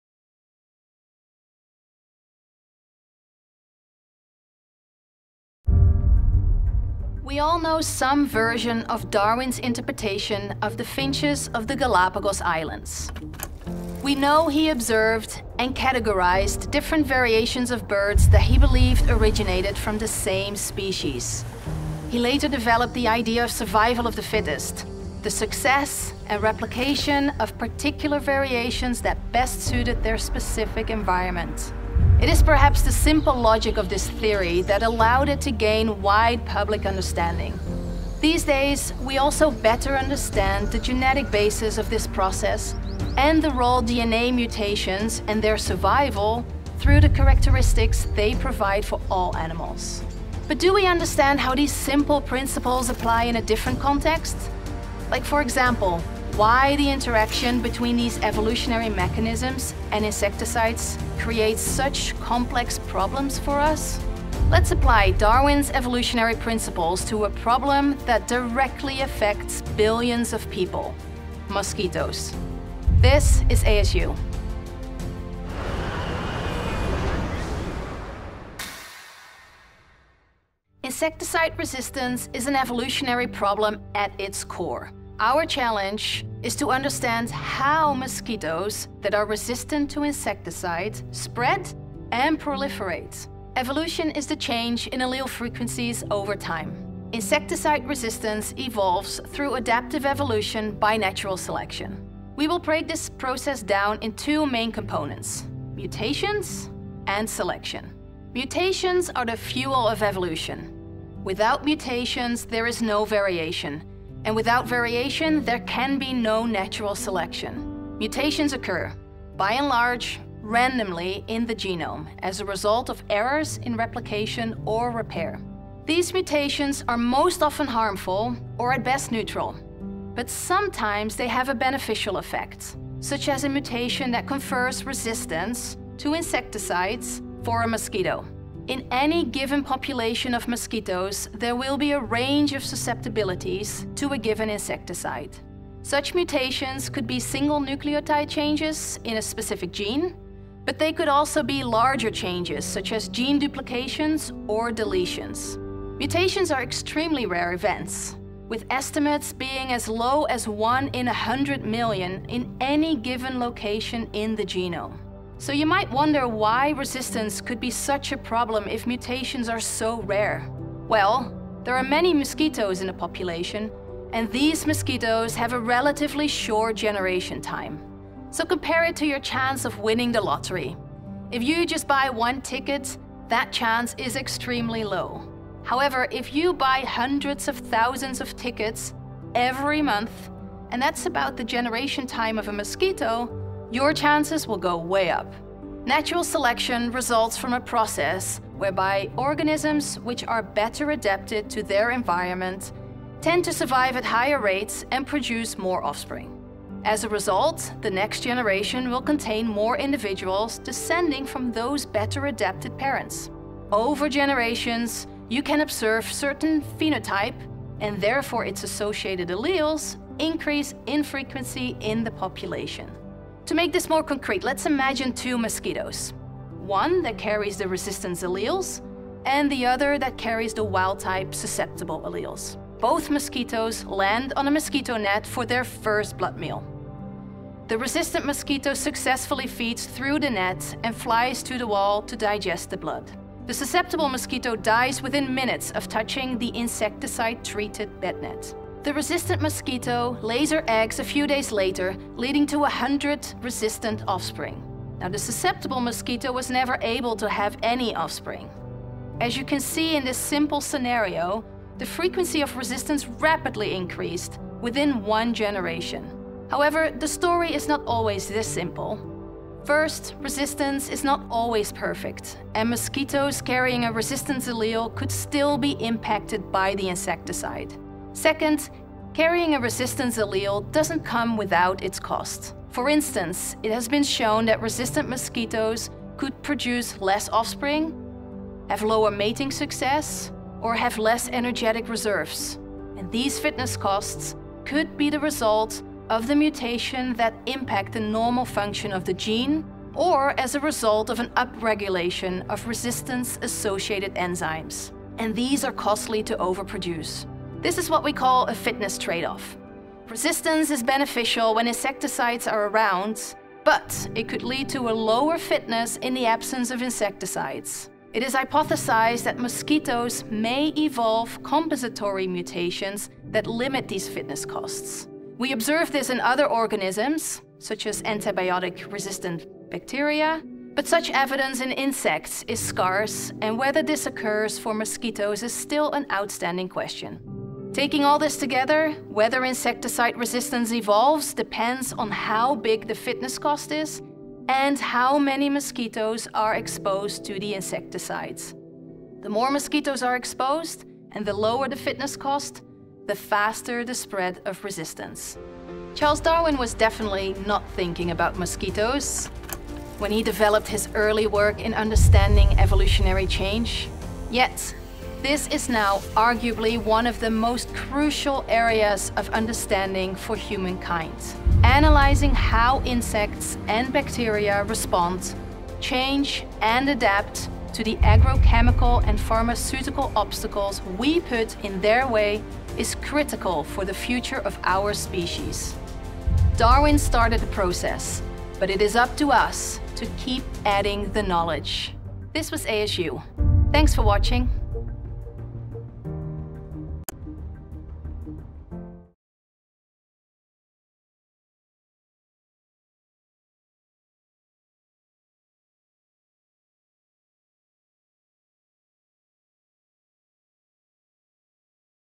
Insecticide Resistance and Evolution
How does Darwin's theory of evolution help explain how insects, like mosquitos, become resistant to insecticides? Biologist Silvie Huijen talks about how evolution can impact the effect of insecticides over time.

00:00 Introduction
01:20 What Darwin's theory of evolution tells us about insecticide resistance.
02:09 How insecticide resistance works.
03:26 Phenotype
04:32 Resistance allele